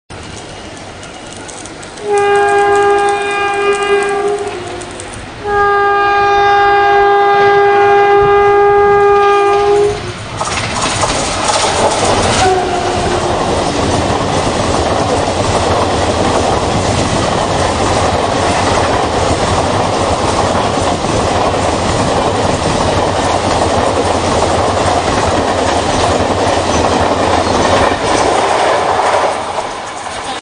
A VERY VERY FAST GUJARAT EXPRESS
9012 Gujarat Express with a BL WCAM1# 21844 rushes through Saphale station in full fury. It was really a great sight to see a WCAM1 aggressively doing above 100kph.
Video by Arzan Kotval